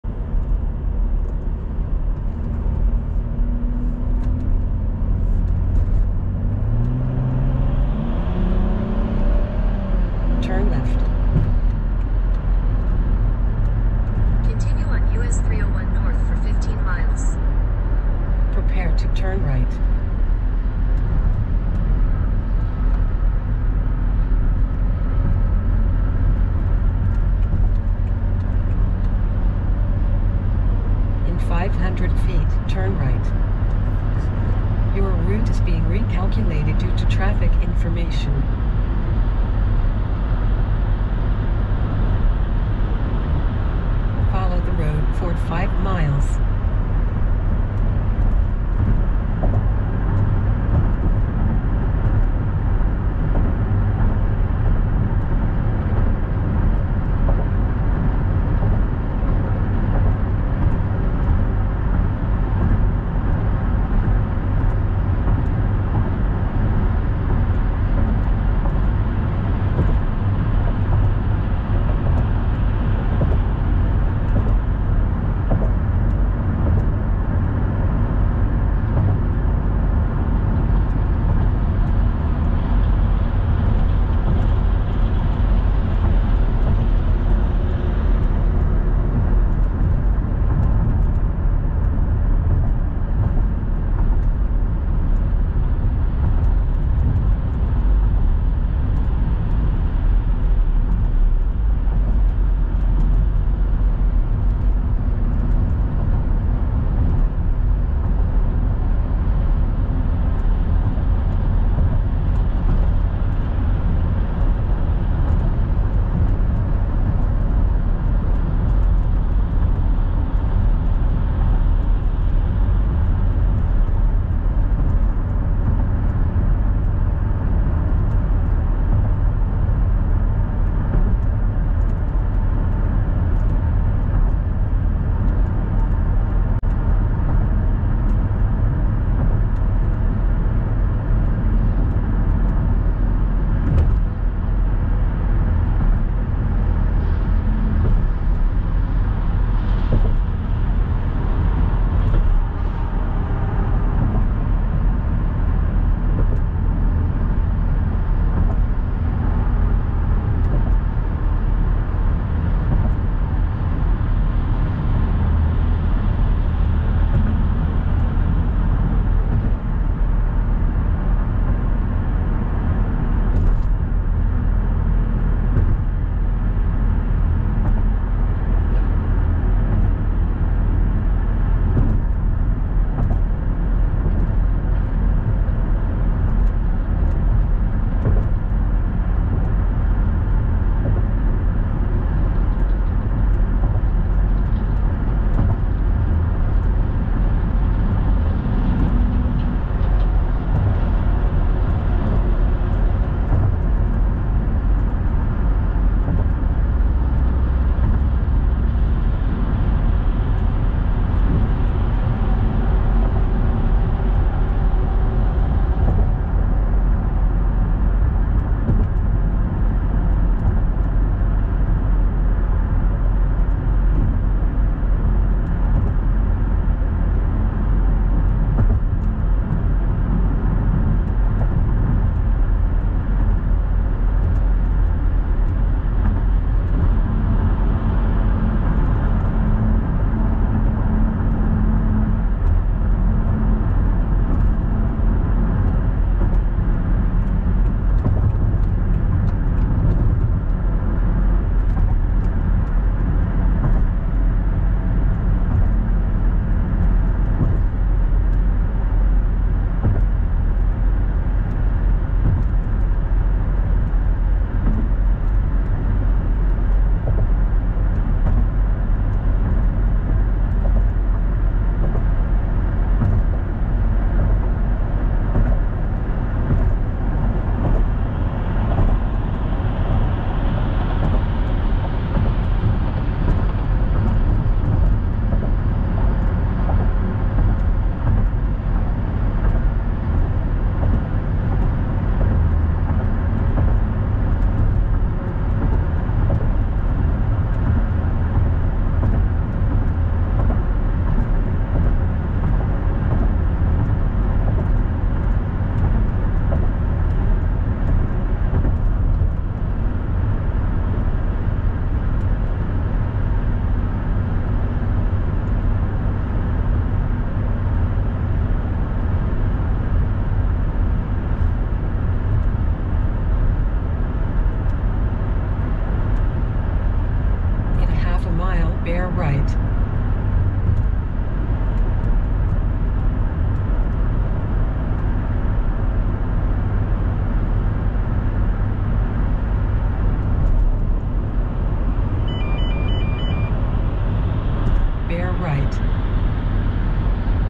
Driving the Chesapeake Bay Bridge
Driving eastbound on the Chesapeake Bay Bridge towards the Eastern Shore of Maryland